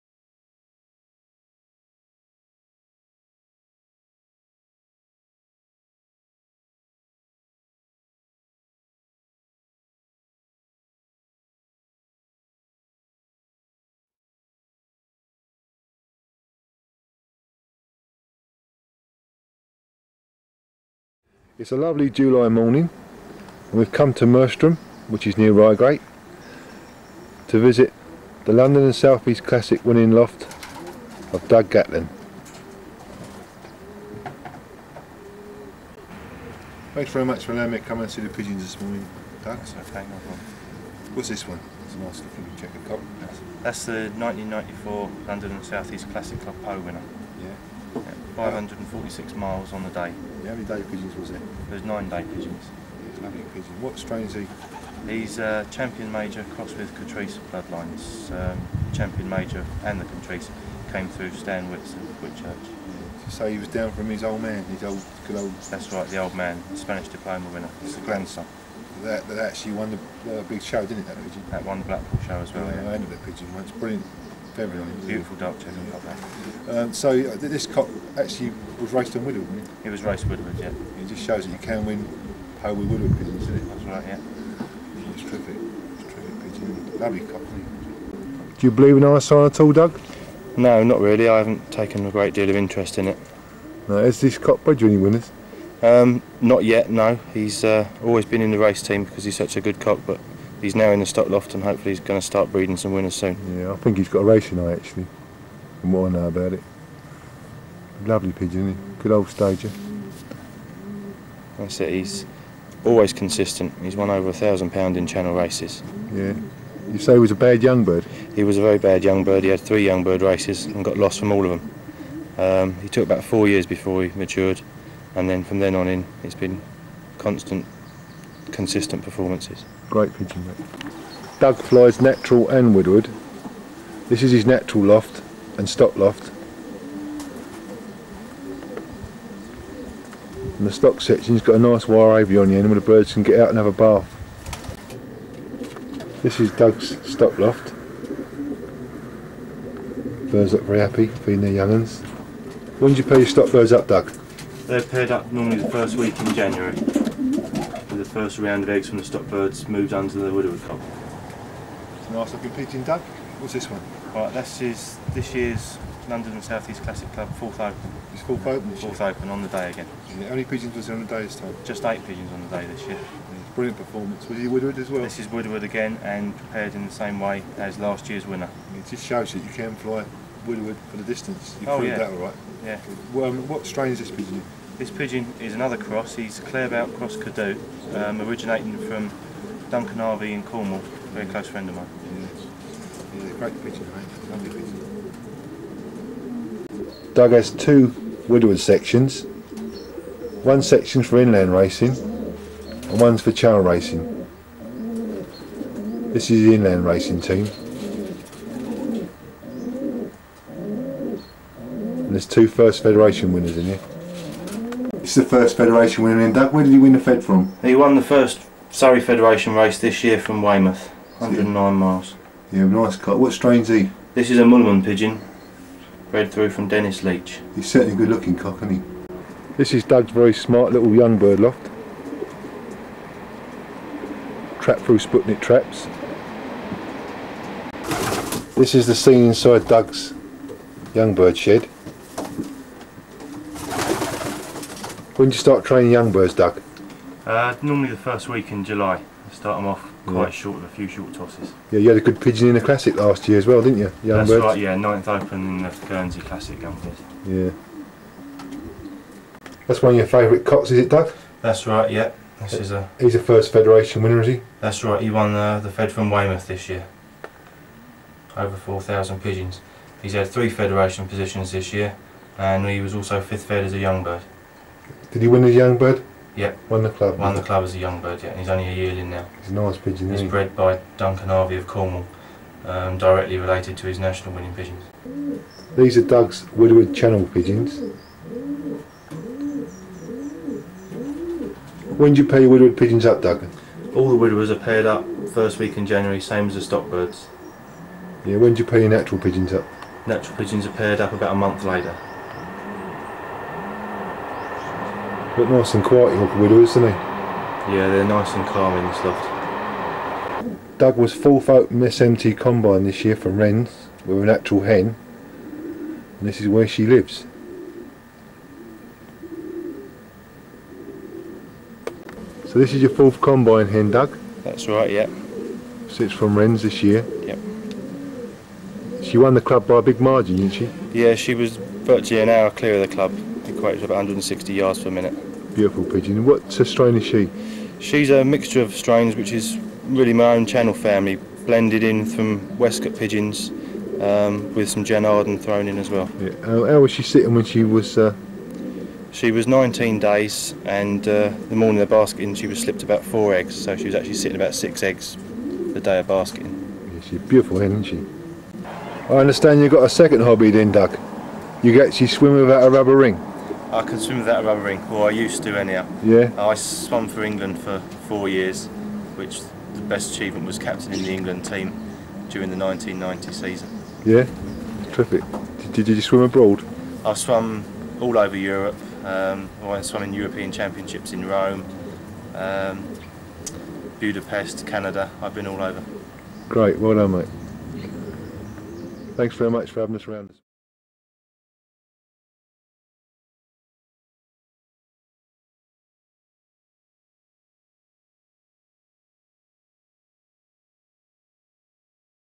Video 491: Doug Gatland of Mersham: Premier Pigeon Racer
Doug Gatland of Mersham: Doug only had a small loft set at his home in Surrey, but was very successful with his pigeons, winning the Federation many times and 1st open L&SECC Pau (550 miles). He was also a champion swimmer and swam for the England national team at venues all over the world. A really great loft visit. (July 2021)